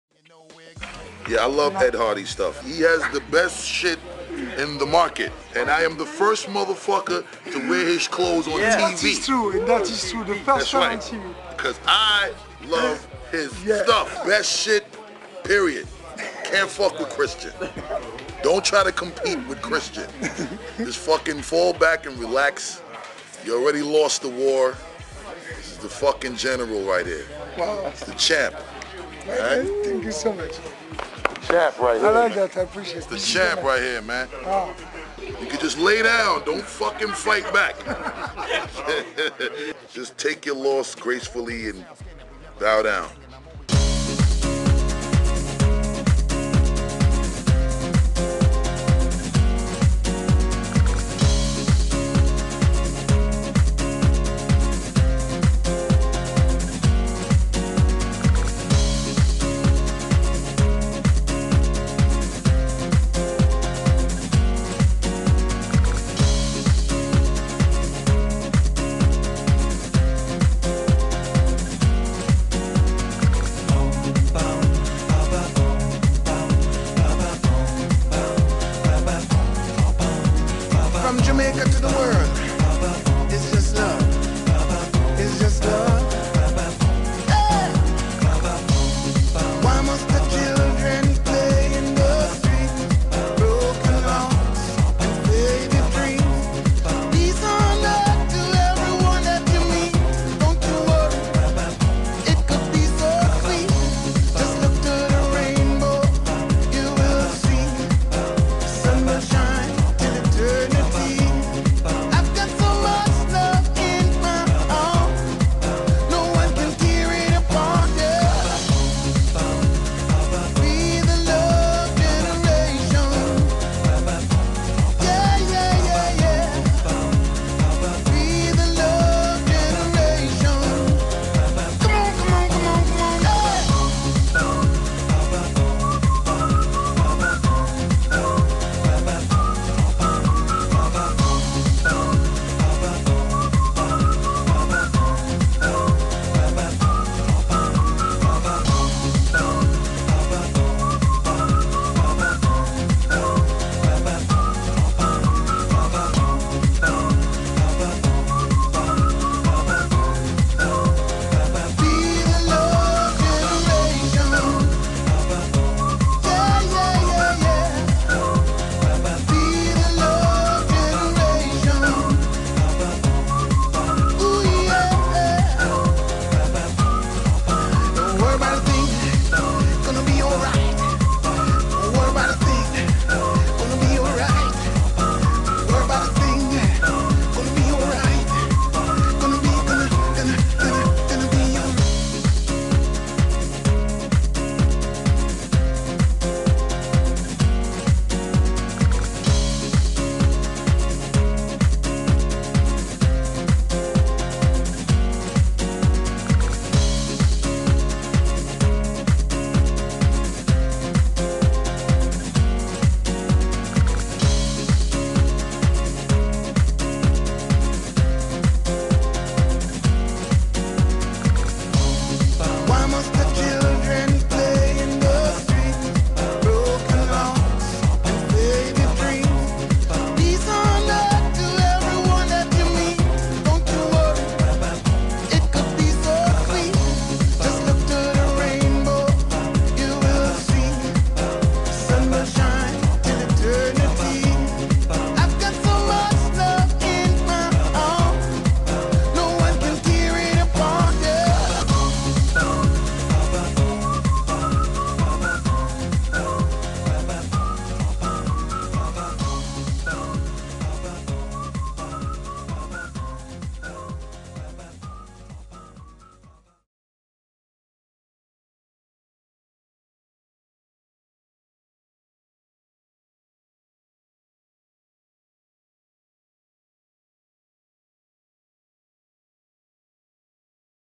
Busta Rhymes and Christian Audigier the General in the World of Fashion
Busta busts some compliments to Christian like never before for the Ed Hardy Gear